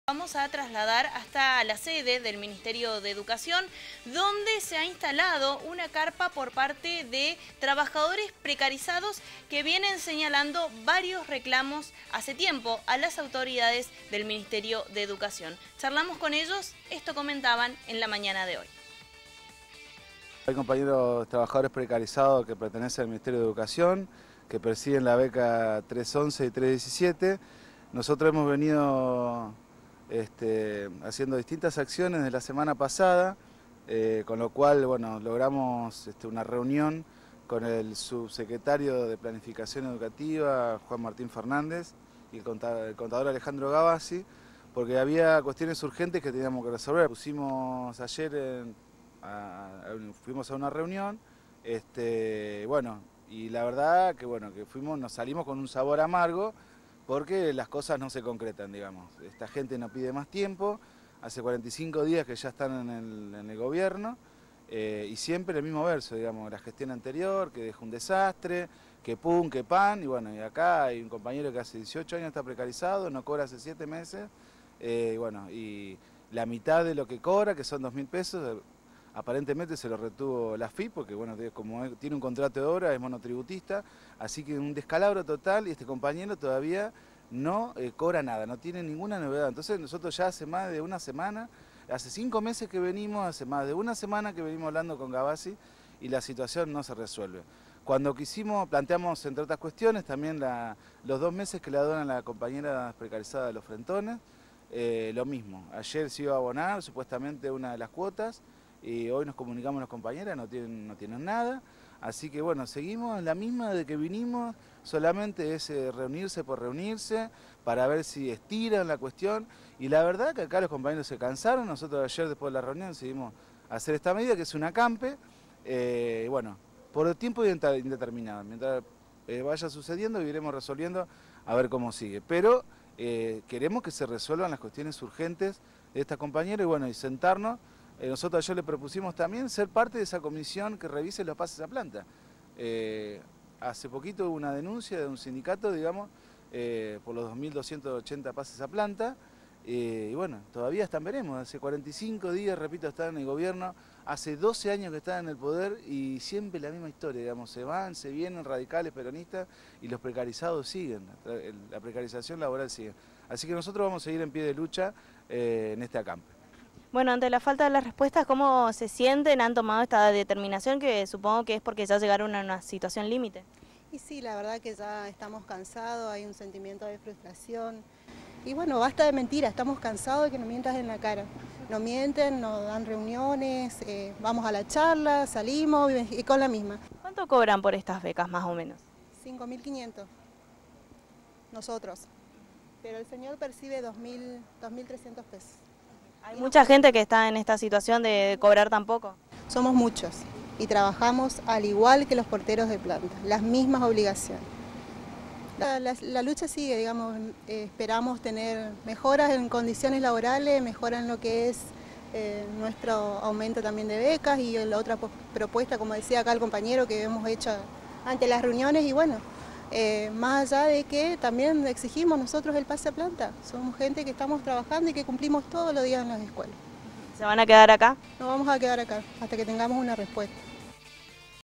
ACAMPE DE PRECARIZADOS EN EDUCACIÓN
Trabajadores becados del Ministerio de Educación iniciaron un acampe por diversos reclamos